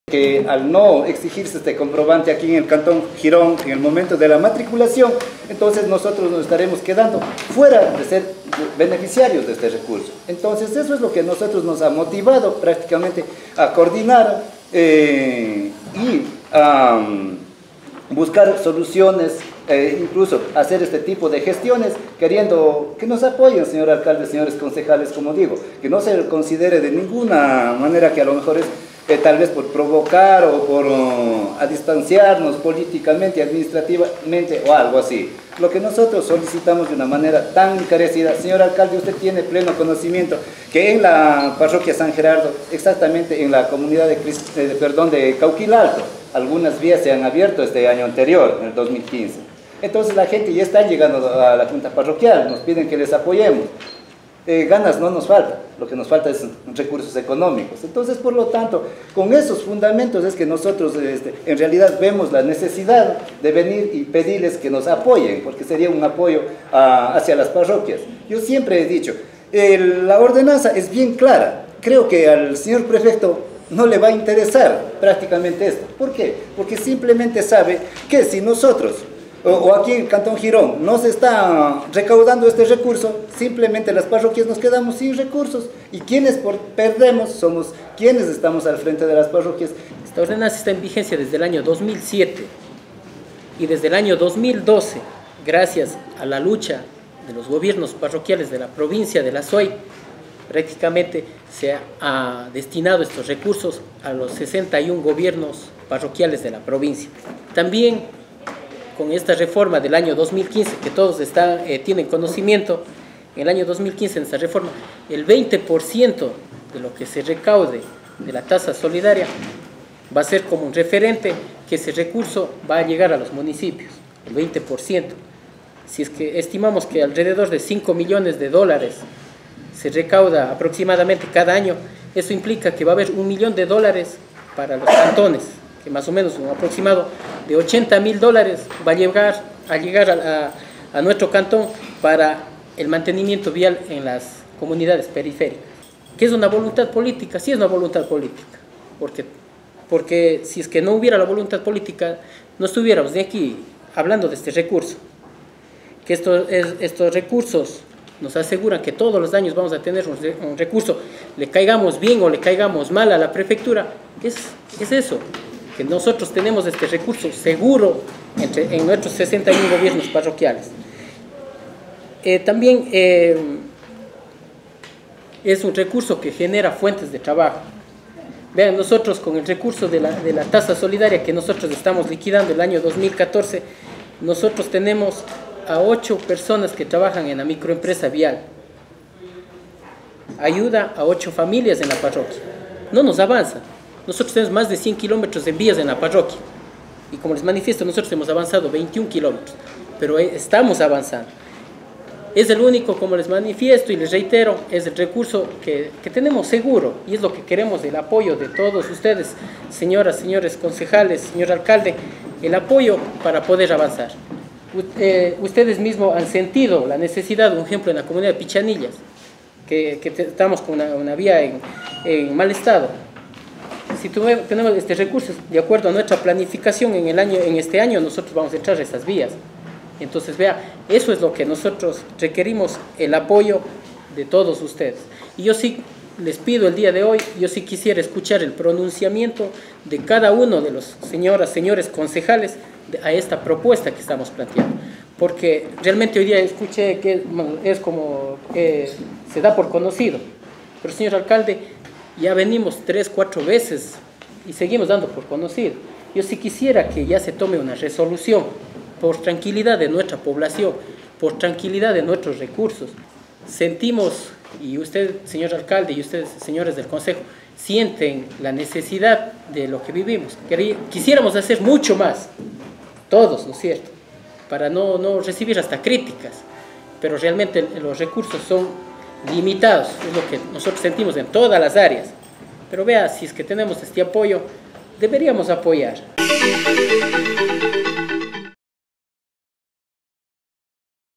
Juntas Parroquiales defienden Tasa Solidaria
En la sesión, los presidentes de las juntas parroquiales expusieron su preocupación y pidieron a concejales y alcalde se tome una resolución de apoyo para el inmediato cobro de la tasa solidaria en Girón, puesto que de no ser así, se corre el riesgo de perder esos recursos.